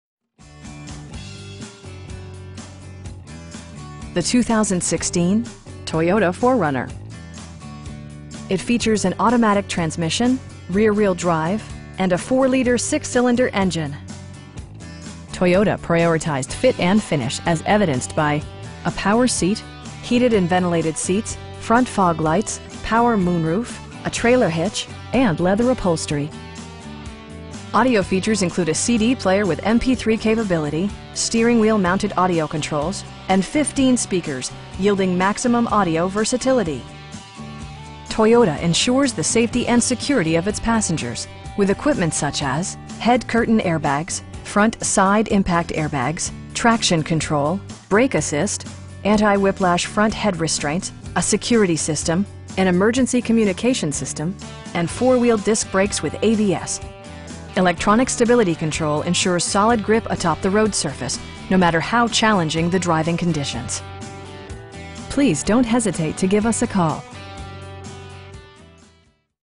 2016 Toyota 4Runner Limited in Lithonia, GA 30038
The 2016 Toyota 4Runner. It features an automatic transmission, rear-wheel drive, and a 4 liter 6 cylinder engine. Toyota prioritized fit and finish as evidenced by: a power seat, heated and ventilated seats, front fog lights, power moon roof, an overhead console, a trailer hitch, and leather upholstery. Audio features include a CD player with MP3 capability, steering wheel mounted audio controls, and 15 speakers, providing excellent sound throughout the cabin. Toyota ensures the safety and security of its passengers with equipment such as: head curtain airbags, front SIDE impact airbags, traction control, brake assist, anti-whiplash front head restraints, a security system, an emergency communication system, and 4 wheel disc brakes with ABS. Electronic stability control ensures solid grip atop the road surface, no matter how challenging the driving conditions. We pride ourselves in the quality that we offer on all of our vehicles. Please don't hesitate to give us a call.